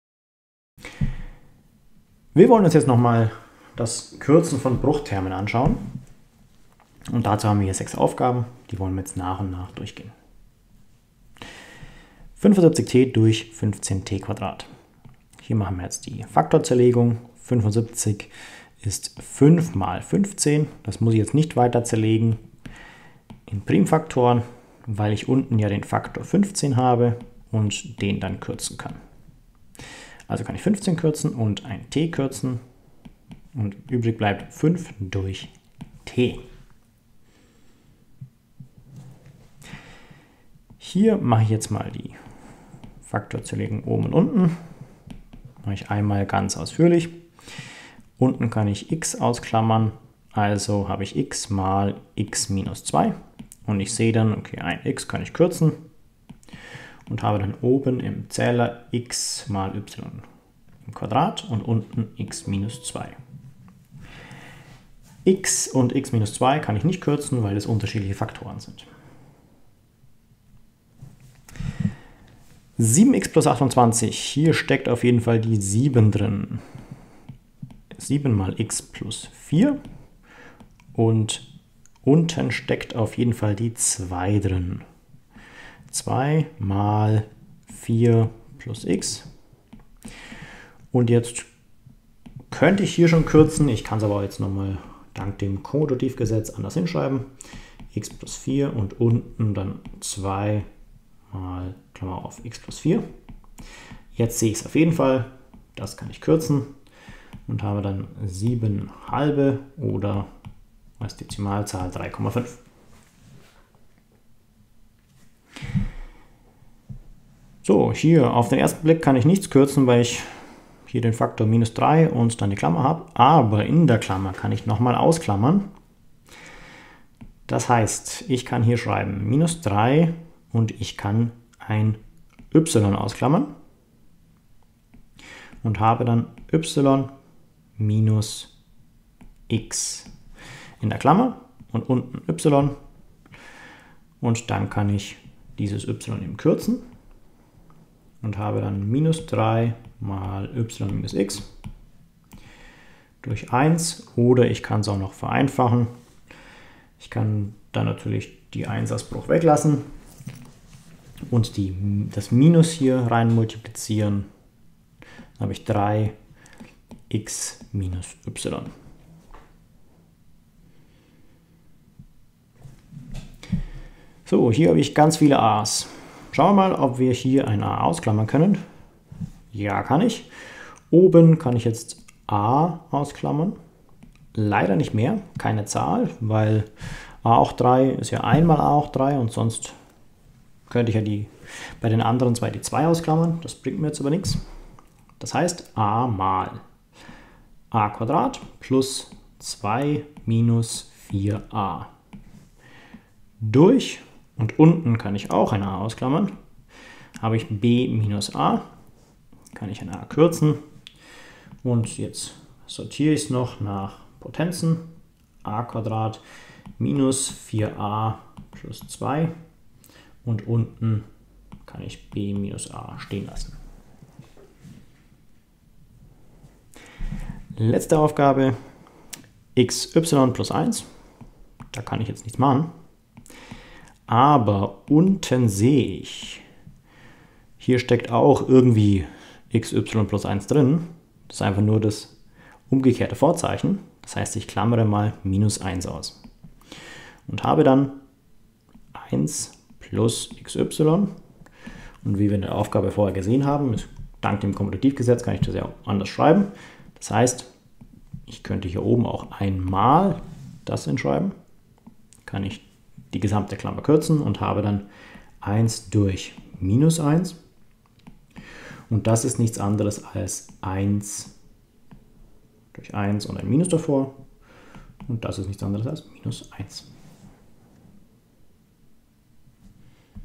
Übung 1- Bruchterme kürzen - Daheim bei Heim
Übungsaufgaben zum Kürzen von Bruchtermen